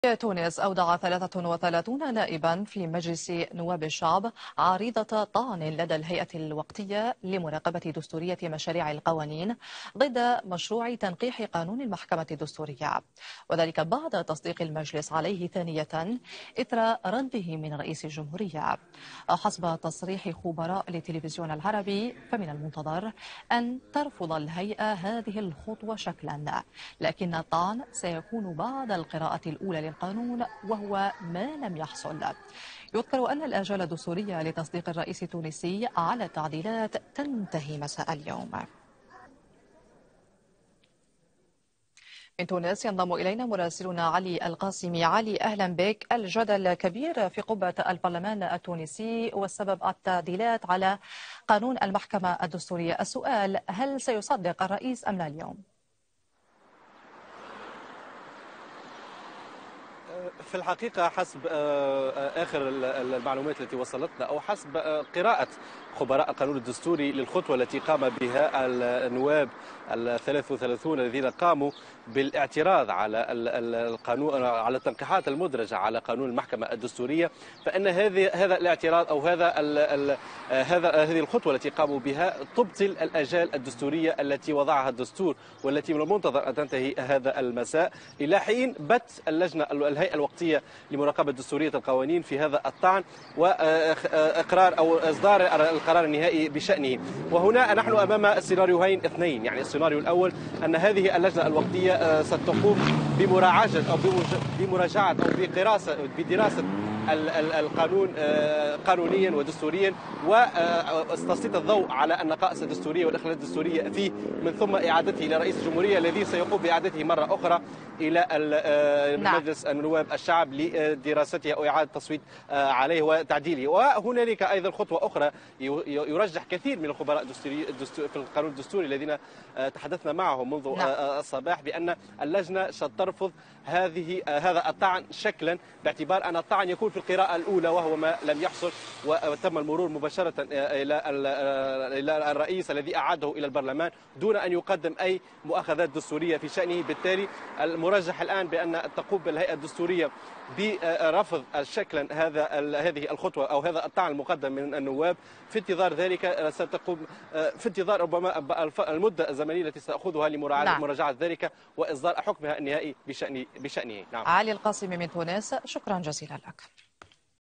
برلمانيون تونسيون يقدمون عريضة طعن في مشروع تنقيح قانون المحكمة الدستورية │ العربي اليوم
تقدم أكثر من 30 نائبا في البرلمان التونسي بطعن ضد مشروع القانون الذي أقره المجلس في الرابع من الشهر الجاري والخاص بتنقيح قانون المحكمة الدستورية.